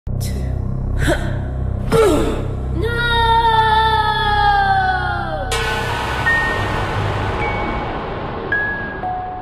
Liveleak: Little girl dies horribly, falls breaks back blood splatter gore
Me vs life

fuck you dont you dare read this